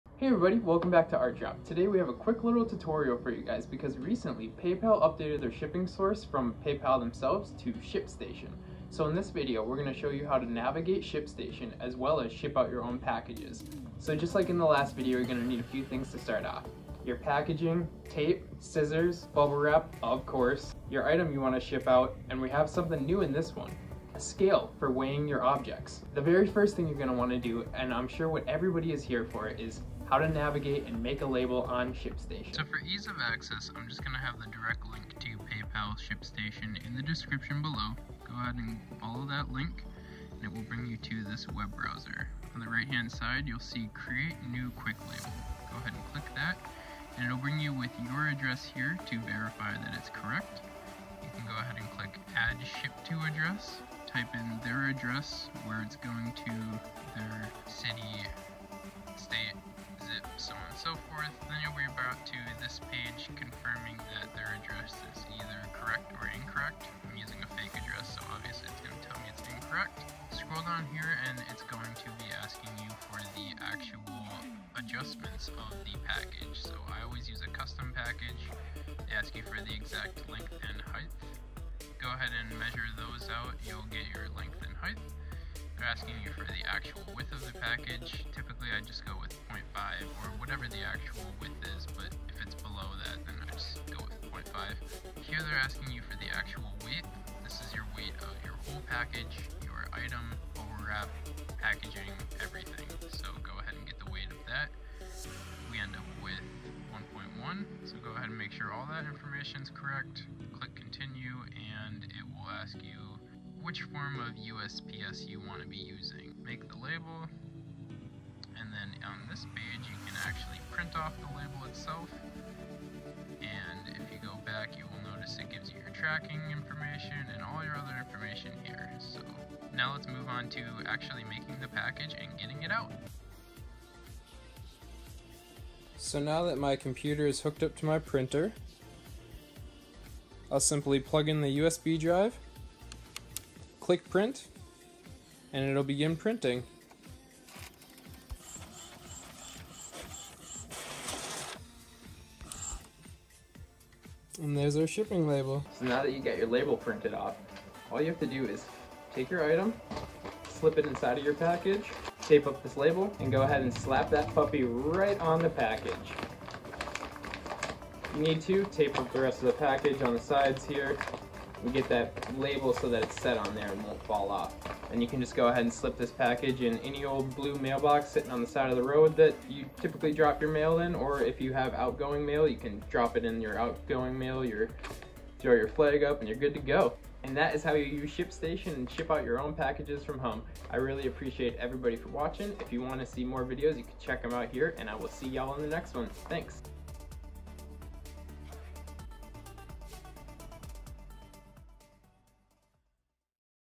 *Updated* How to Ship your Own Packages from Home Using Shipstaion via Paypal
This video is a updated how to video about shipping your own packages from home using the new  Shipstation in PayPal! These steps will help you ship your own package from home instead of having to go to the post office. I hope you find it useful and enjoy!

This is my old video from 4 years ago:
which you guys gave so much love and found so useful! thank you so much and like last time feel free to ask questions below and I will try to help!

SHOW LESS